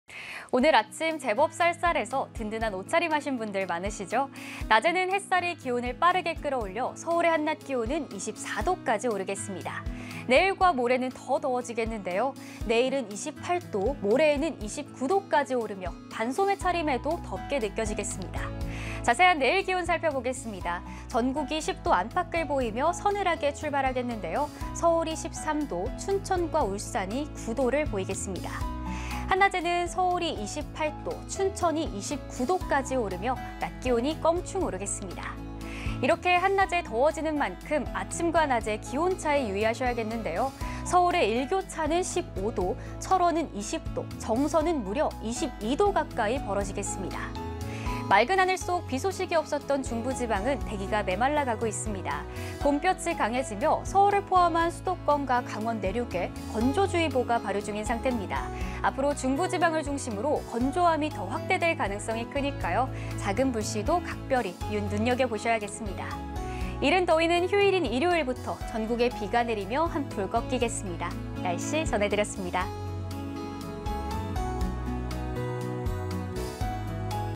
[날씨] 낮 기온 점점 올라요…큰 일교차 유의 / 연합뉴스TV (YonhapnewsTV)
[날씨] 낮 기온 점점 올라요…큰 일교차 유의

오늘 아침 제법 쌀쌀해서, 든든한 옷차림 하신 분들 많으시죠?

낮에는 햇살이 기온을 빠르게 끌어올려, 서울의 한낮 기온은 24도까지 오르겠습니다.

내일과 모레는 더 더워지겠는데요.

내일은 28도, 모레에는 29도까지 오르며 반소매 차림에도 덥게 느껴지겠습니다.

자세한 내일 기온 살펴보겠습니다.

전국이 10도 안팎을 보이며 서늘하게 출발하겠는데요.

서울 13도, 춘천과 울산이 9도를 보이겠습니다.

한낮에는 서울이 28도, 춘천이 29도까지 오르며 낮 기온이 껑충 오르겠습니다.

이렇게 한낮에 더워지는 만큼, 아침과 낮의 기온차에 유의하셔야겠는데요.

서울의 일교차는 15도, 철원이 20도, 정선은 무려 22도까지 벌어지겠습니다.

맑은 하늘 속 비 소식이 없었던 중부지방은 대기가 메말라가고 있습니다.

봄볕이 강해지며 서울을 포함한 수도권과 강원내륙에 '건조주의보'가 발효 중인 상태입니다.

앞으로 중부지방을 중심으로 건조함이 확대될 가능성이 크니까요.

작은 불씨도 각별히 눈여겨보셔야겠습니다.

이른 더위는 휴일인 일요일부터 전국에 비가 내리며 한풀 꺾이겠습니다.

(박서정 기상캐스터)